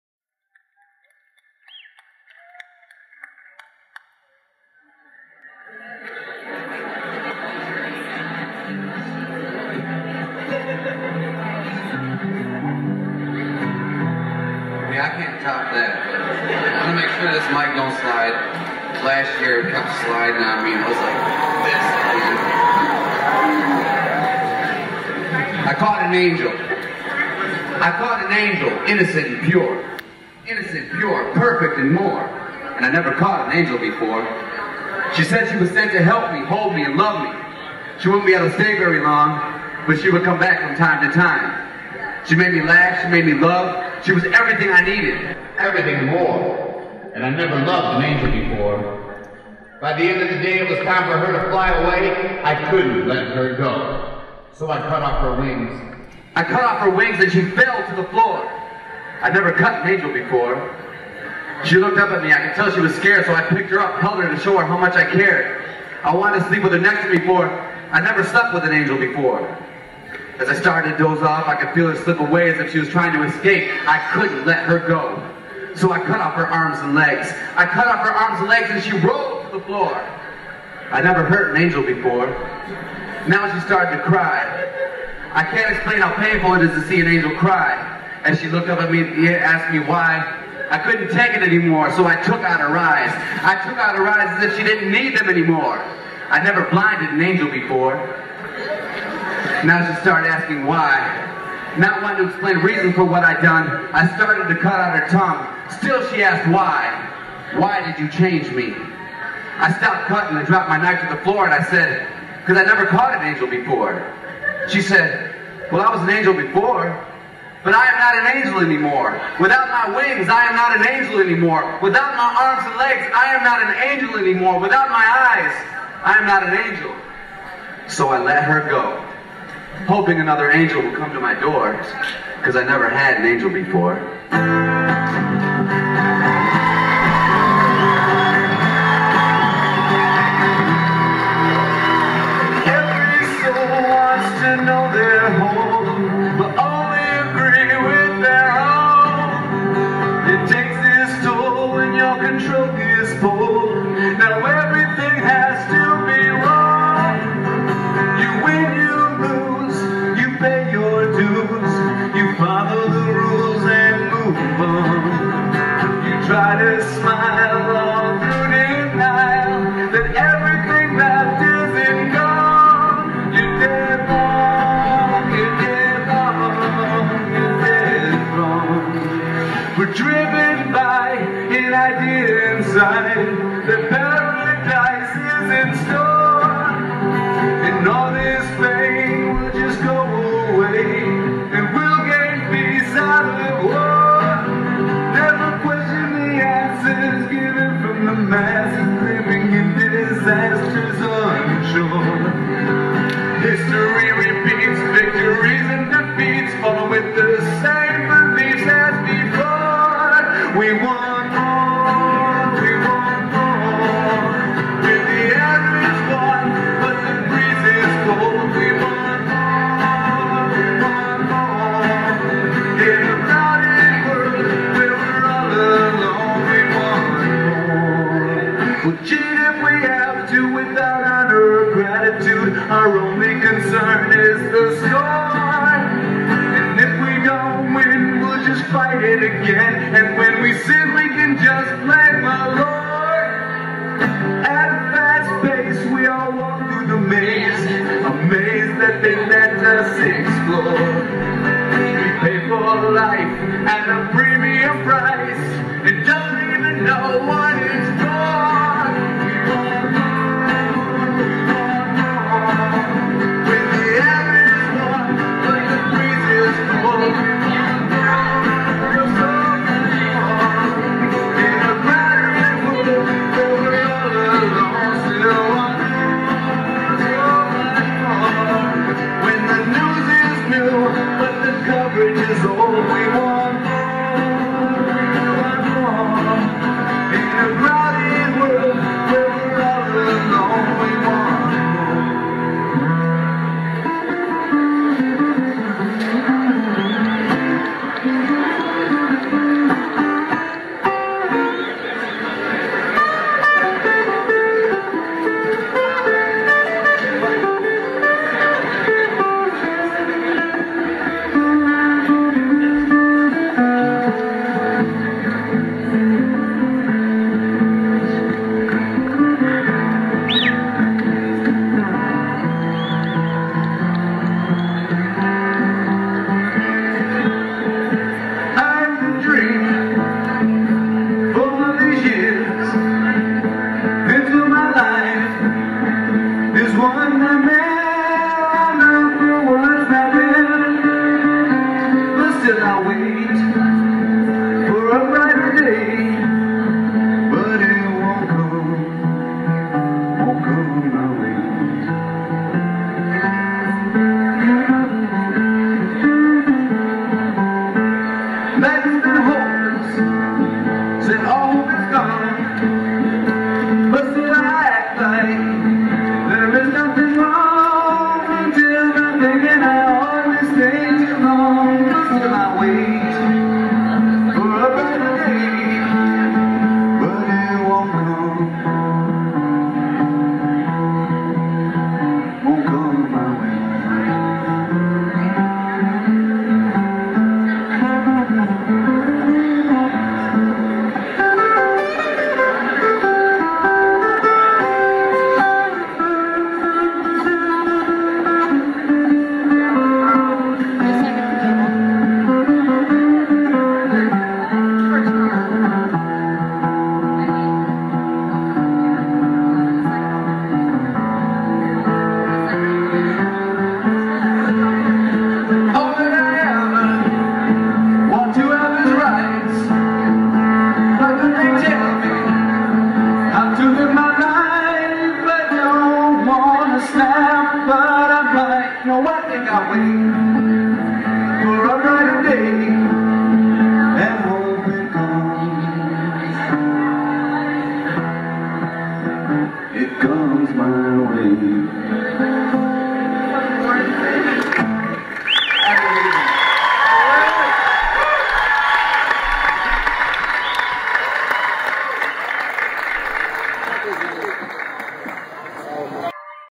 39th Annaul Wheatland Festival 2012 Nakkim of Numedia
nakkim of numedia playing at wheatland bluegrass music doing his style of folk